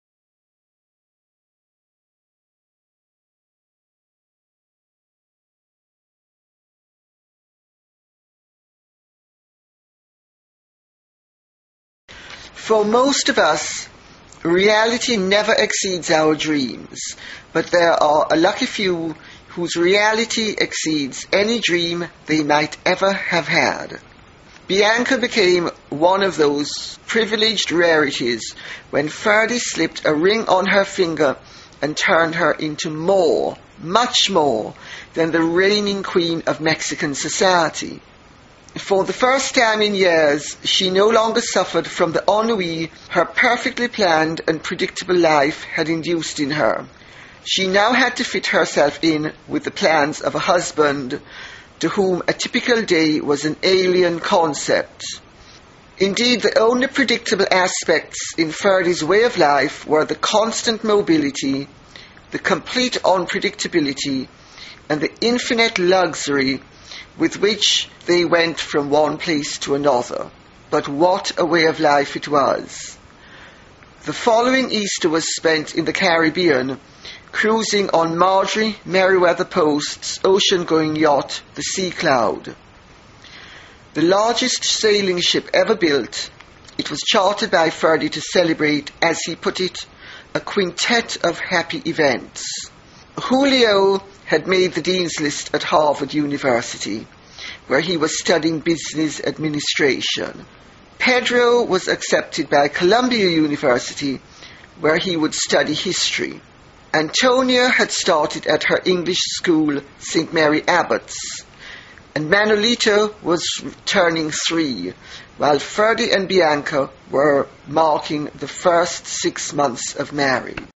Empress Bianca
Lady Colin Campbell reads from her novel "Empress Bianca" during her appearance at the Parker Ladd Author Breakfast at Cafe Boulud in the Brazilian Court hotel on Dec. 19, 2008.